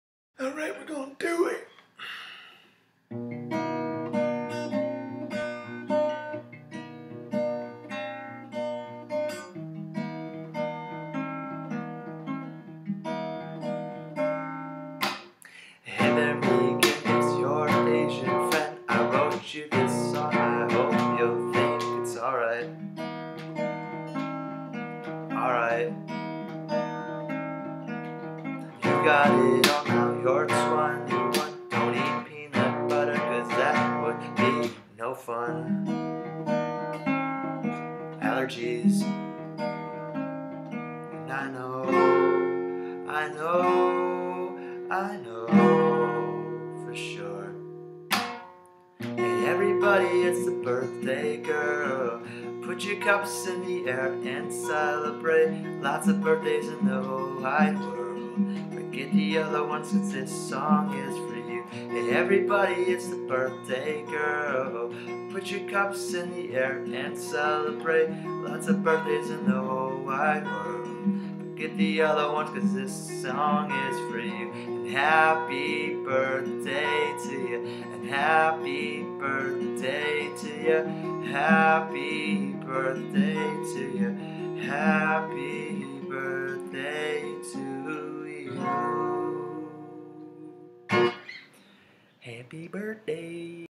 Meegan steals peanut butter
Return of the corny birthday song.

Lyrics:
Hey there Meegan
It’s your asian friend
I wrote you a song
I hope you’ll think it’s alright, alright

You’ve got it all
Now you’re twenty one
Don’t eat peanut butter
Cause that would be no fun
Allergies

I know, I know, I know for sure

Hey everybody it’s the birthday girl
Put your cups in the air and celebrate
Lotsa birthdays in the whole wide world
Forget the other ones cause this song is for you